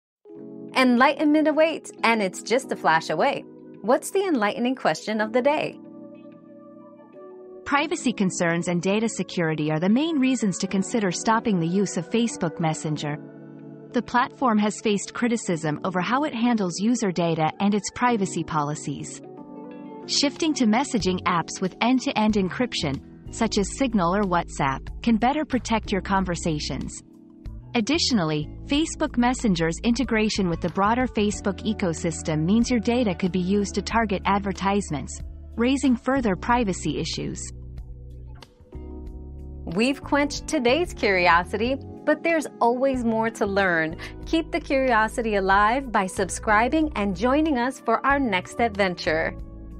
Why should you stop using Facebook Messenger?
Why You Should Ditch Facebook Messenger 👉 Ditch Messenger 👉 Learn about the privacy concerns and data security issues with Facebook Messenger, and discover why switching to messaging apps like Signal or WhatsApp can better protect your conversations and safeguard your privacy.

Laura S. Harris (2024, April 13.) Why should you stop using Facebook Messenger?
    🌐 AskAbout.video